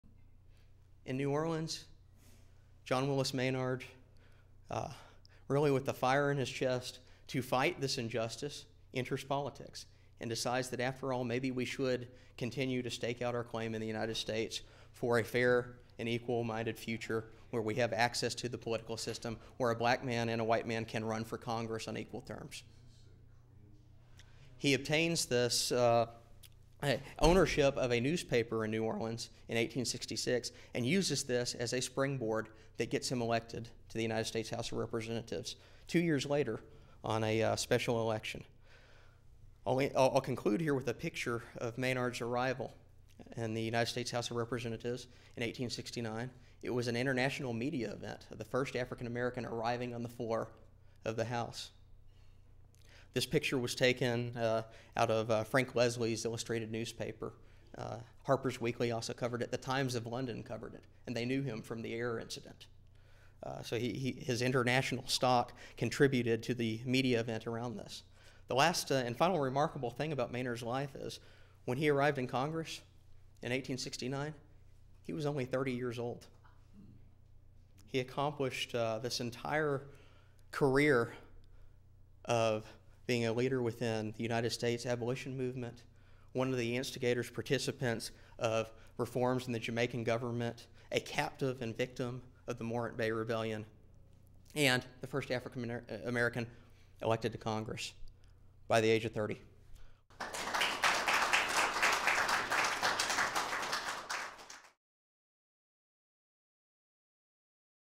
Career of Abolitionist John Willis Menard - Preview
Menard was elected to the House of Representatives from Louisiana in 1868 but because of a contest did not serve.  He did make the first speech on the House floor by an African American, however; pleading his case for office.